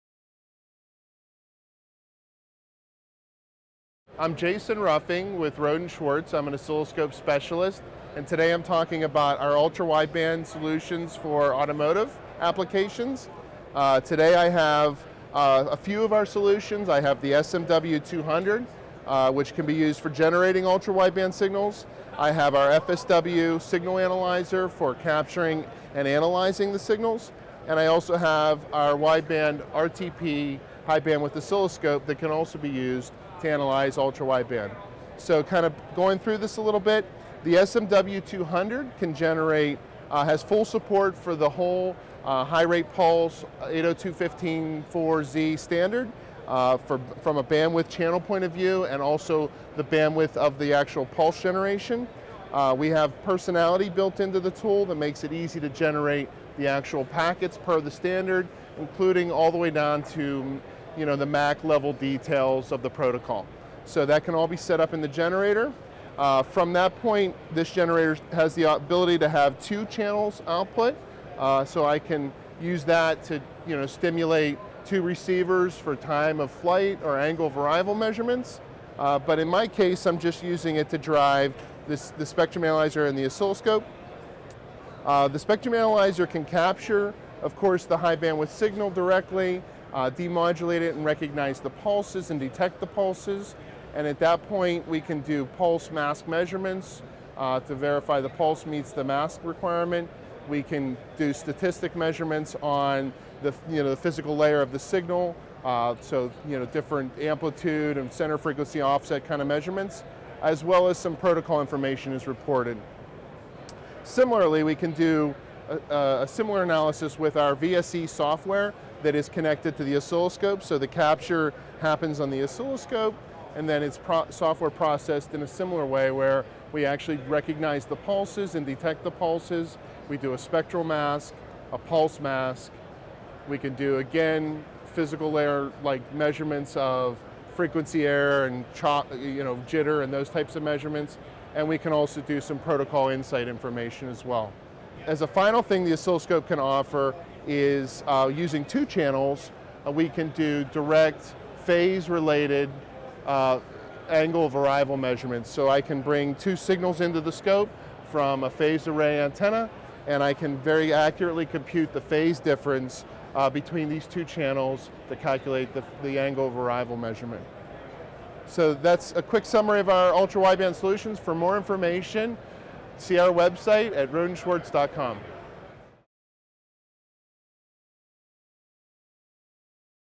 Ultra-Wideband (UWB) for Automotive Applications
Rohde & Schwarz test solutions enable accurate test and verification of UWB devices in all phases of the product lifecycle.